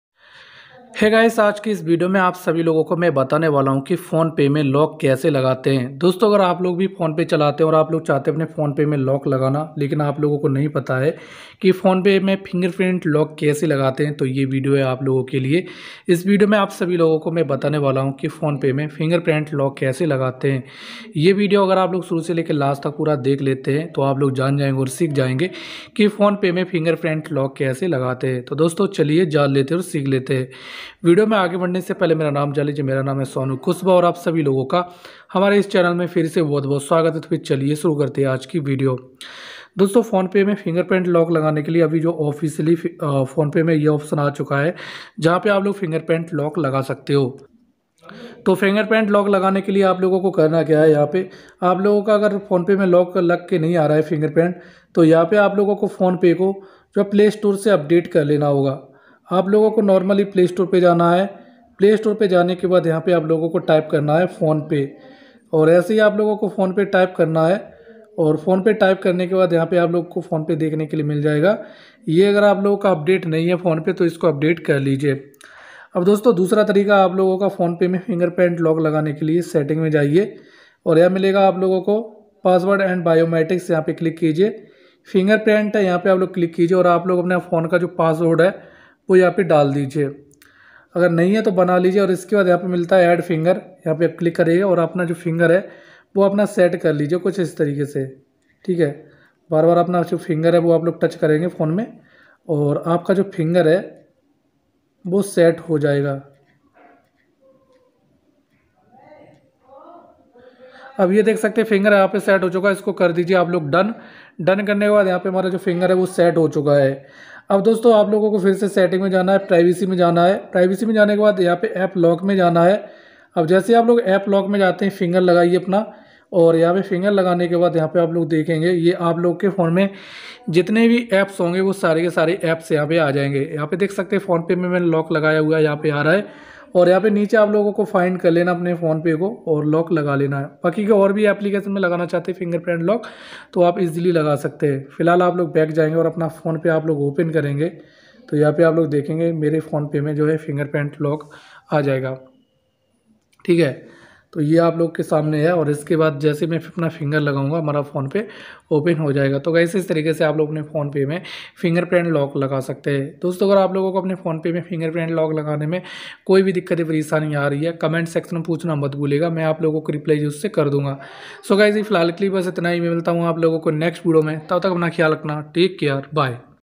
Phonepe me fingerprint lock kaise lagayen | How To Set Fingerprint Lock in Phonepe
Hello dosto is video me maine bataya hai ki phonepe me fingerprint lock kaise lagate hain